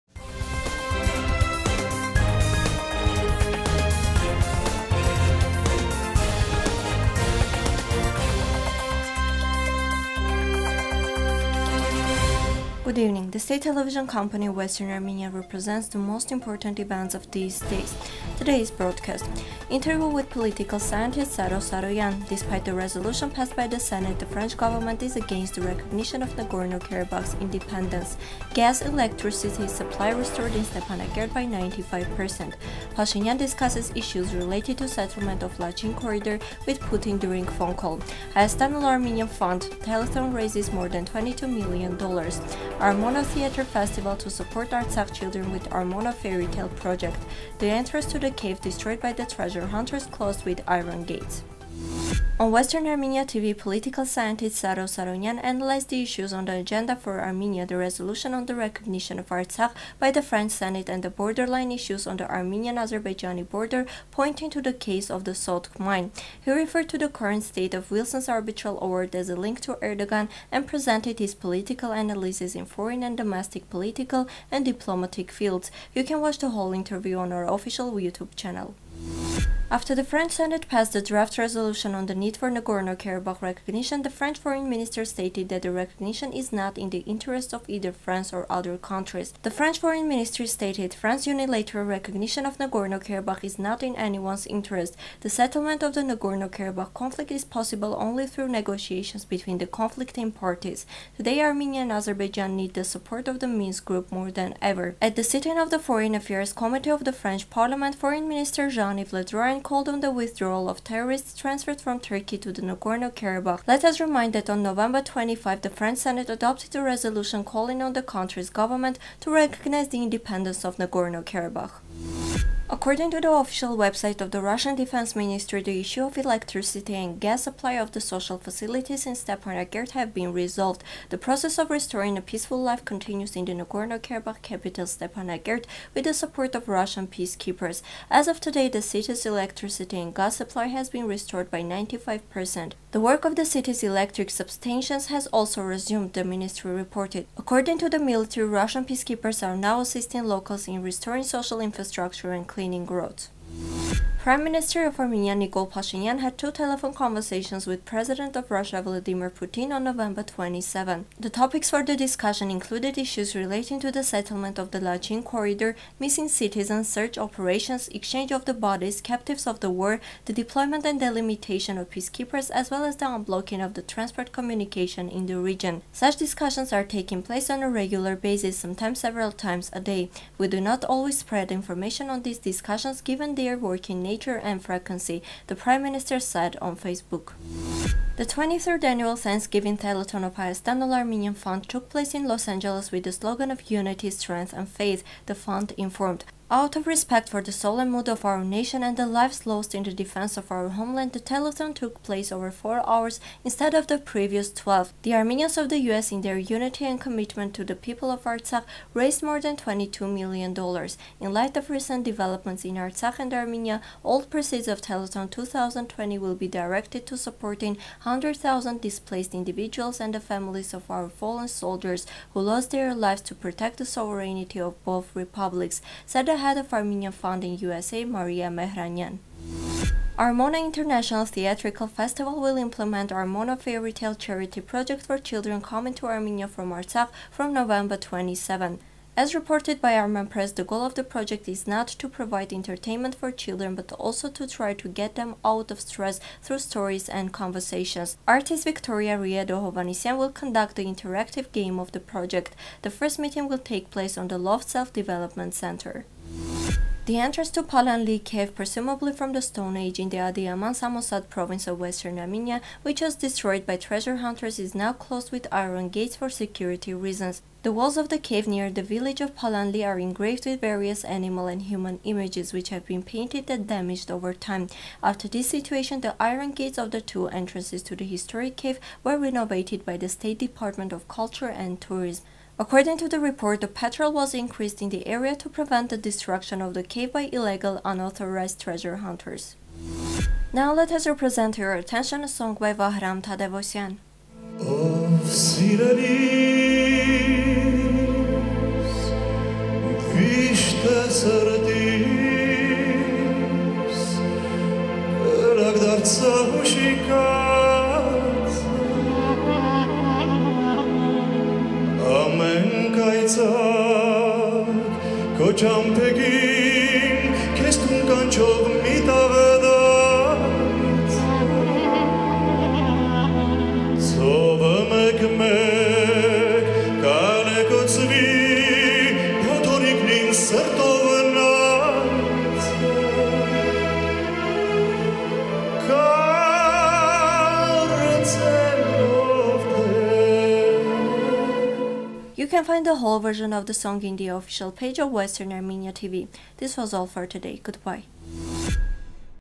Hayastan All Armenian Fund Telethon raises $22,990,898.NEWS 27 .11.2020
1.Interview with political scientist Saro Saroyan
2.Despite the resolution passed by the Senate, the French government is against the recognition of Nagorno-Karabakh's independence
3.Gas, electricity supply restored in Stepanakert by 95-97%
4.Pashinyan discusses issues related to settlements of Lachin corridor with Putin during phone call
5.Hayastan All Armenian Fund Telethon raises  $22,990,898.
6."Armmono" theater festival to support Artsakh children with "Armmono-Fairy Tale" project
7.The entrance to the cave destroyed by the treasure hunters closed with iron gates